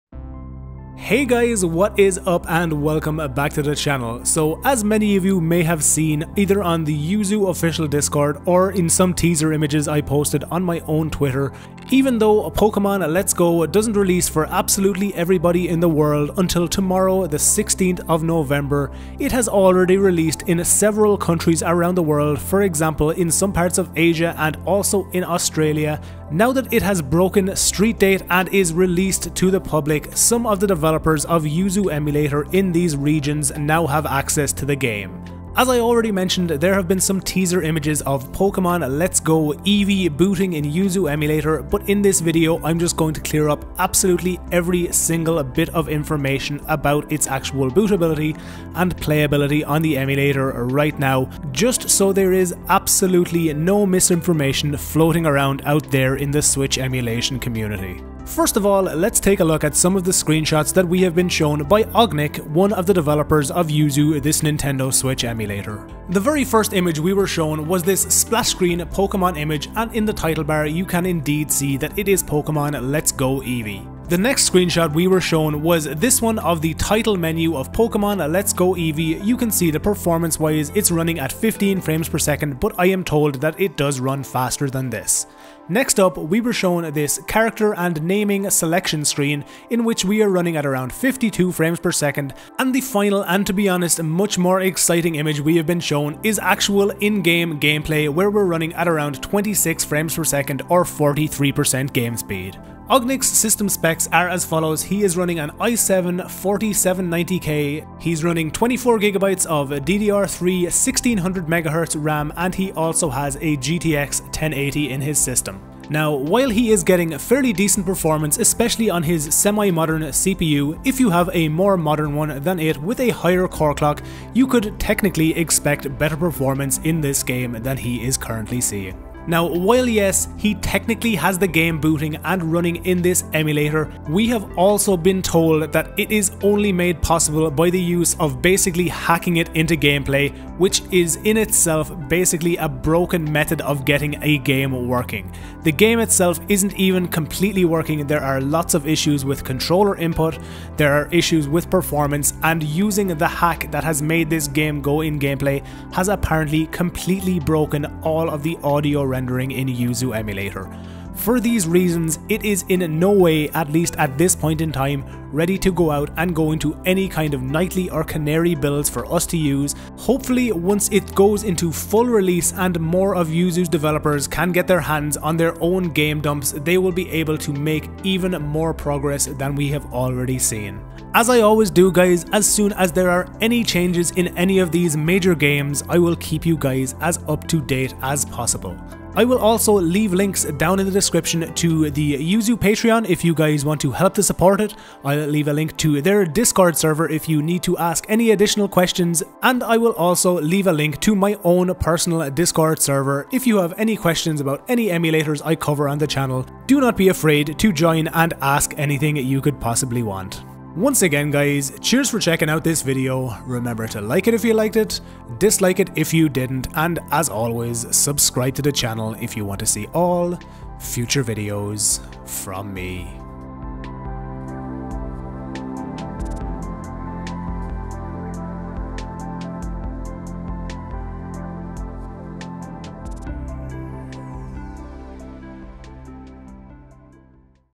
Pokemon Let's Go Booting in Yuzu Emulator? | Everything We Know So Far
Is Pokemon Let's Go Eevee/Pikachu going to boot and be playable on release day? Lets take a look at all the information

► My PC Specs:
16gb DDR4 3200Mhz RAM
GTX980ti 6gb
256gb NVME M.2 SSD